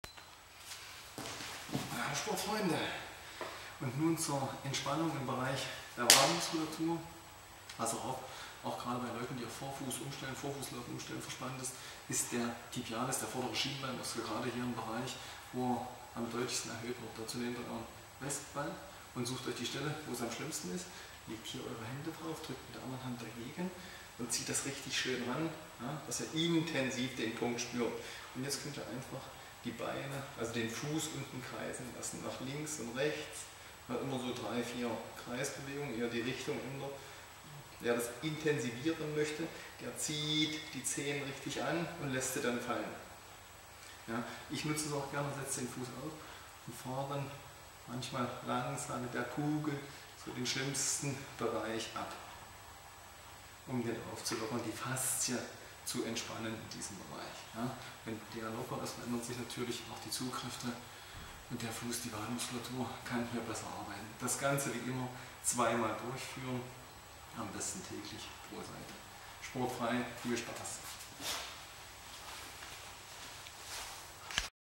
wesp sei stark Faszienlockerung für die Wade tibiales
Diese Übung ist zur Lockerung des m. tibialis, des vorderen Schienbeinmuskels. Gerade bei Vorfußläufern und bei der Umstellung auf den Vorfußlauf ist dieser leicht überlastet und die faszien in dem Bereich verkleben schnell.